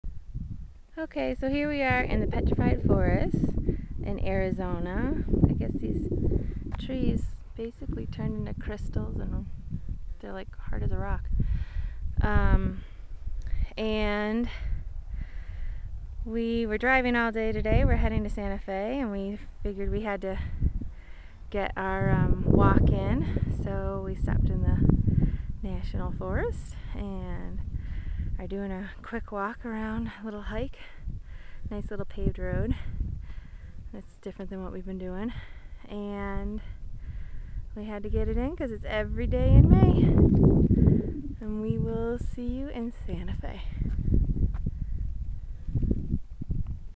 Petrified Forest
Stopped at the Petrified Forest for Every Day in May.  For more information on this fitness challenge go